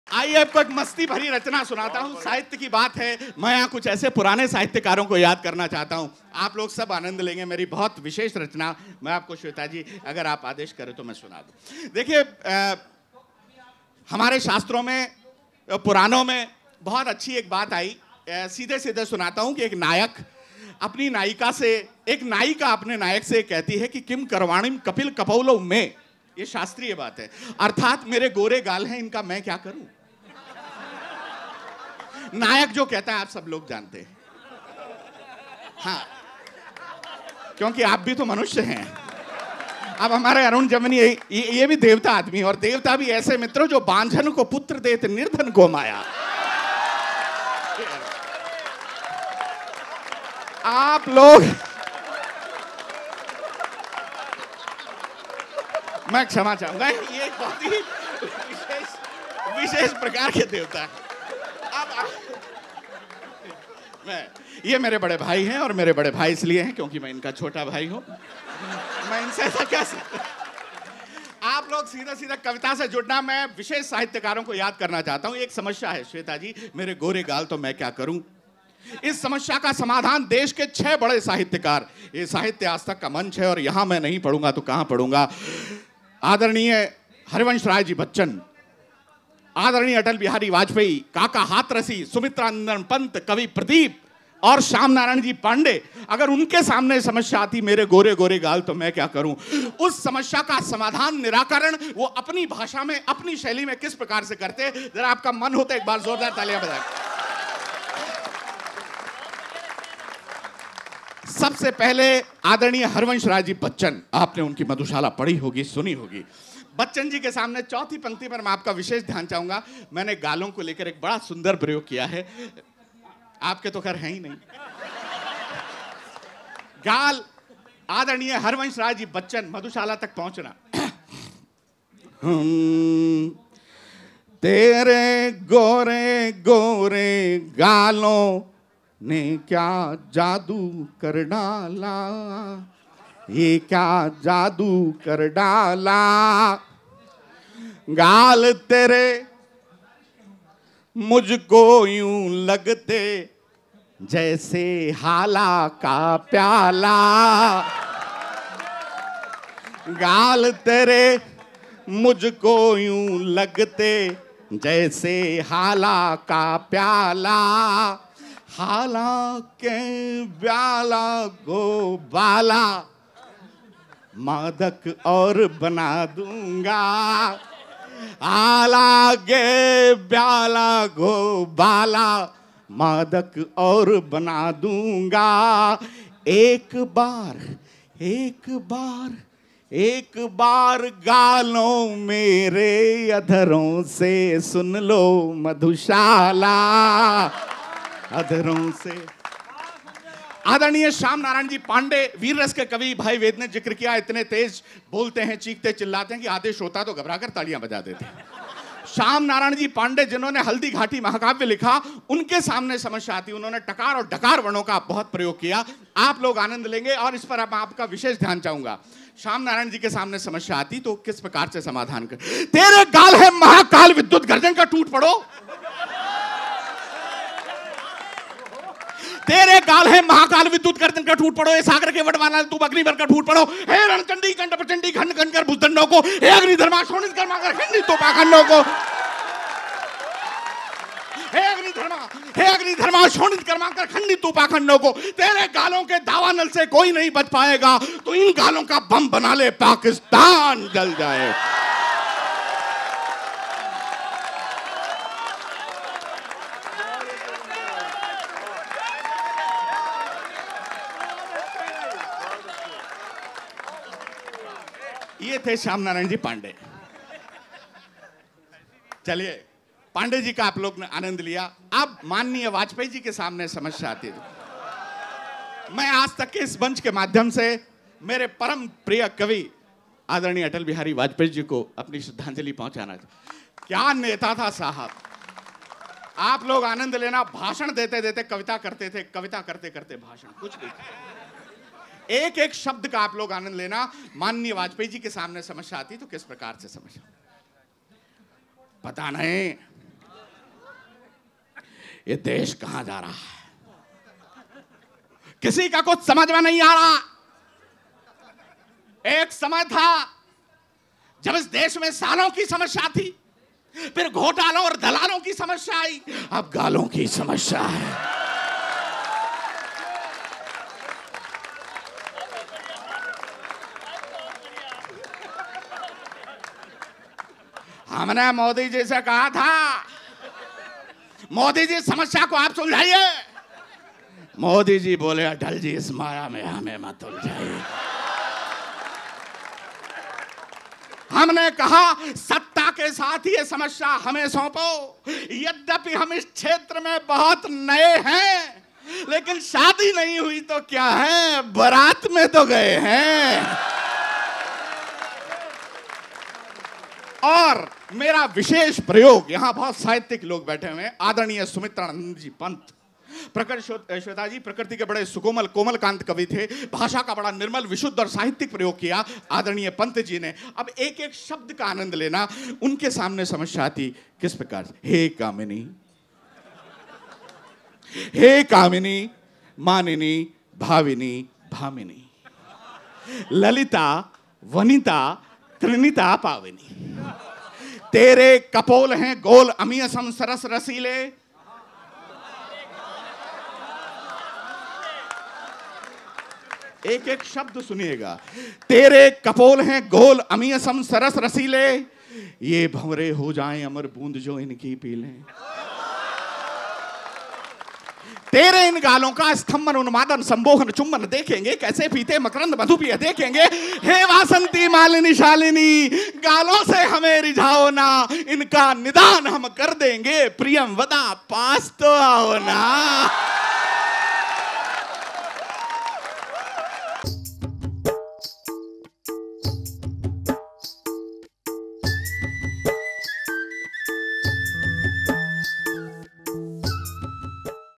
गालों की समस्या पर जब फंसे PM Modi | मेरे गोरे-गोरे गाल मैं क्या करूं | Sanjay Jhala | Sahitya Tak
हास्य कवि संजय झाला ने हरिवंश राय बच्चन, अटल बिहारी वाजपेयी, सुमित्रा नंदन पंत, श्यामनारायण पांडेय और पीएम मोदी के बहाने से एक मस्ती भरी रचना सुनाई. नायक और नायिका के बीच की बातचीत को उन्होंने जिस अंदाज में दर्शकों के सामने पेश किया दर्शक लहालोट हो गए. साहित्य तक पर सुनिए यह मजेदार व्यंग्य.